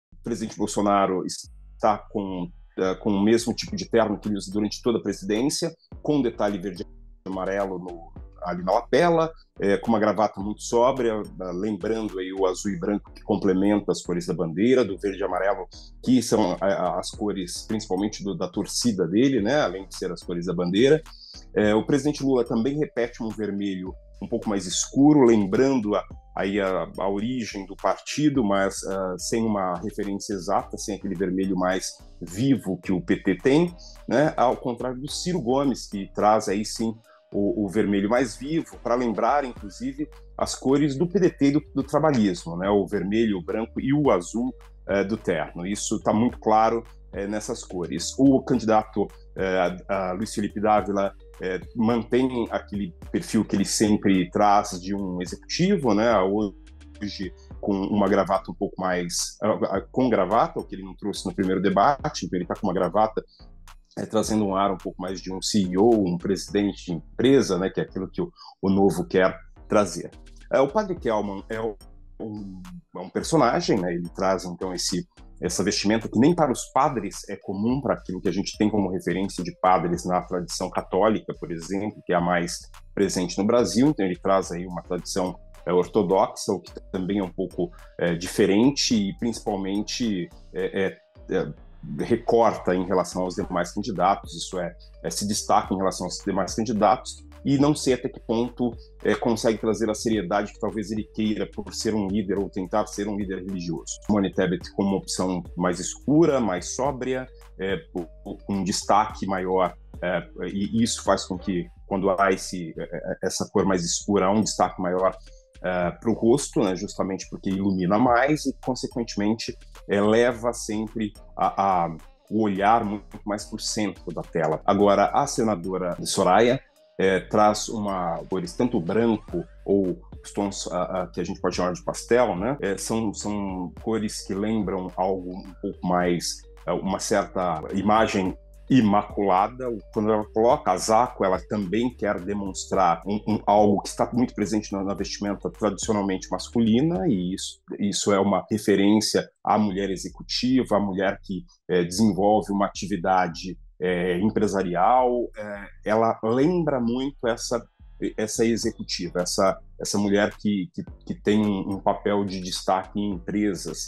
'Dress Code' do debate na Globo: o que a roupa diz sobre os candidatos?
Os presidenciáveis participaram do último debate antes do primeiro turno das eleições. Cientista político avalia o que eles vestiram pode significar para quem assistiu